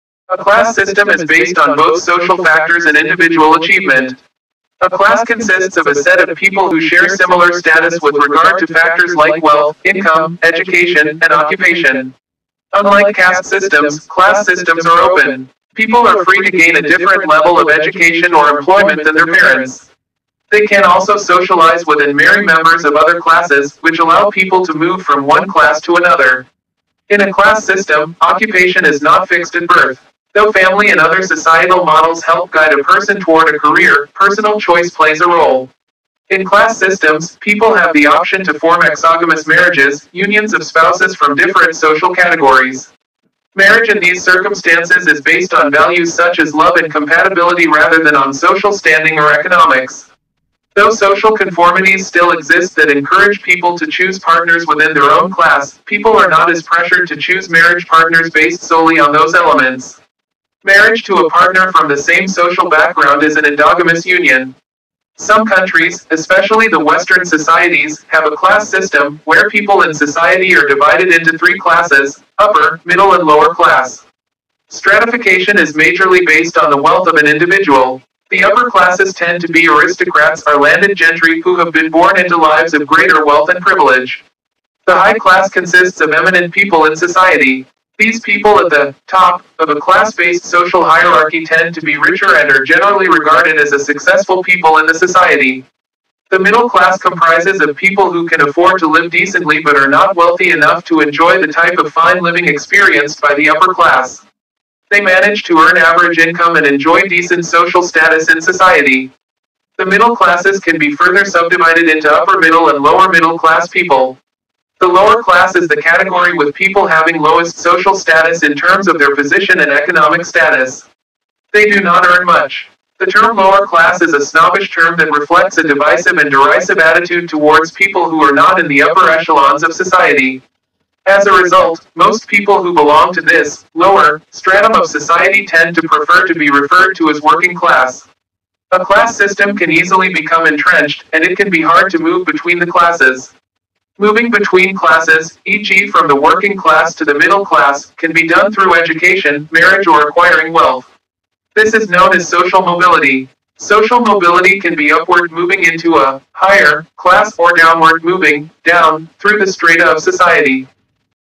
Class System | BG 6th Semester Sociology | Social Stratification | University of Kashmir
A class system is based on both social factors and individual achievement. A class consists of a set of people who share similar status with regard to factors like wealth, income, education, and occupation. Unlike caste systems, class systems are open. People are free to gain a different level of education or employment than their parents. They can also socialize with and marry members of other classes, which allow people to move from one class to another. In a class system, occupation is not fixed at birth.
Though family and other societal models help guide a person toward a career, personal choice plays a role. In class systems, people have the option to form exogamous marriages, unions of spouses from different social categories. Marriage in these circumstances is based on values such as love and compatibility rather than on social standing or economics. Though
social conformities still exist that encourage people to choose partners within their own class, people are not as pressured to choose marriage partners based solely on those elements.

Marriage to a partner from the same social background is an endogamous union. Some countries, especially the western societies, have a class system, where people in society are divided into three classes: upper, middle and lower class. Stratification is majorly based on the wealth of an individual.

The upper classes tend to be aristocrats (or landed gentry) who have been born into lives of greater wealth and privilege. The high-class consists of eminent people in society.
These people at the "top" of a class based social hierarchy tend to be richer and are generally regarded as a successful people in the society.

The middle class comprises of people who can afford to live decently but are not wealthy enough to enjoy the type of fine living experienced by the upper class. They manage to earn average income and enjoy decent social status in society. The middle classes can be further subdivided into upper middle and lower middle class people.

The lower class is the category with people having lowest social status in terms of their position and economic status. They do not earn much. The term lower class is a snobbish term that reflects a divisive and derisive attitude towards people who are not in the upper echelons of society. As a result, most people who belong to this "lower" stratum of society tend to prefer to be referred to as working class.

A class system can easily become entrenched, and it can be hard to move between the classes. Moving between classes – e.g. from the working class to the middle class – can be done through education, marriage or acquiring wealth. This is known as social mobility. Social mobility can be upward (moving into a "higher" class) or downward (moving "down" through the strata of society).